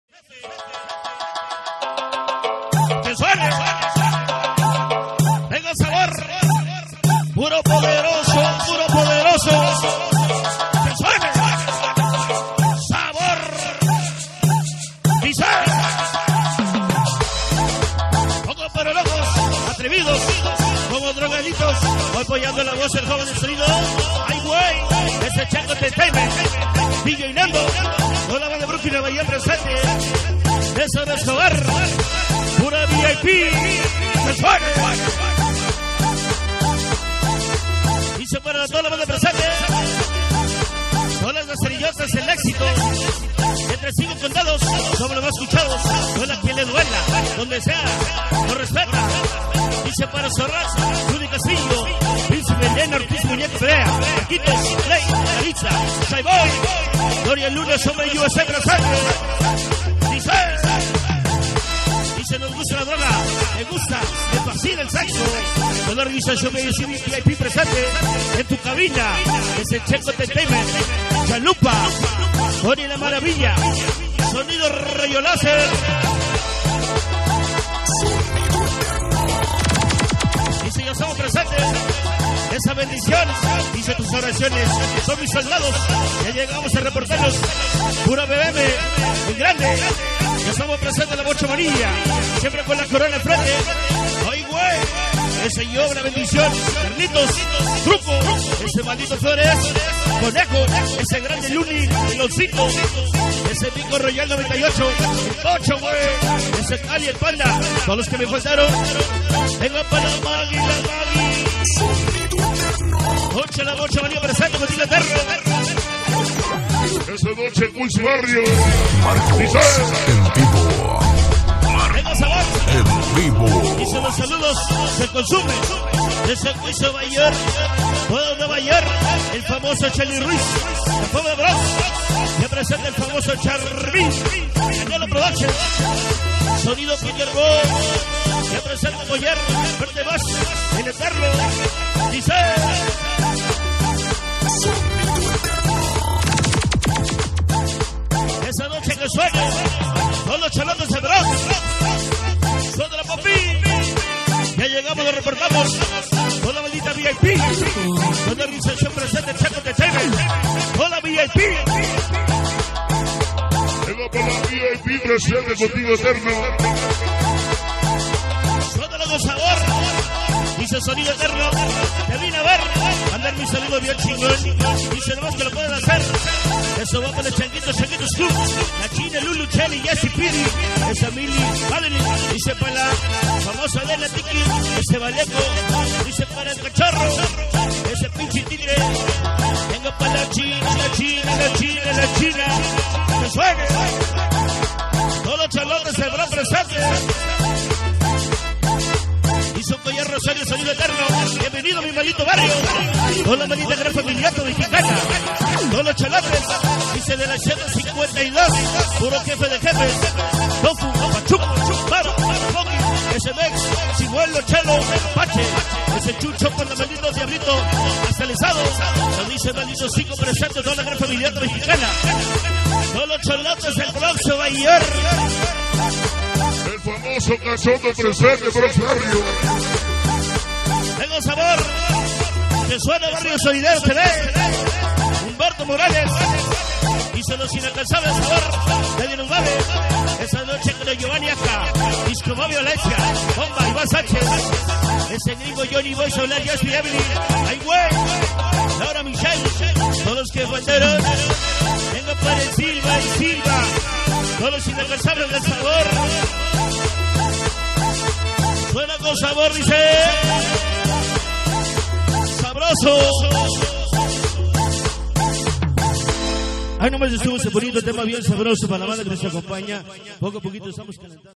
🔥 ASI SE PRESENTO SONIDO ETERNO JUAN HERNANDEZ / EL SONIDO DEL FUTURO / CON UNA CUMBIA SABROSA 2021
Asi Se Presento Sonido Eterno Juan Hernandez " El Sonido Del Futuro " De Passaic New Jersey Con Una Cumbia Sabrosa 2021. Una Cumbia Con Wepa Que Tiene Mucho Ritmo y Sabor.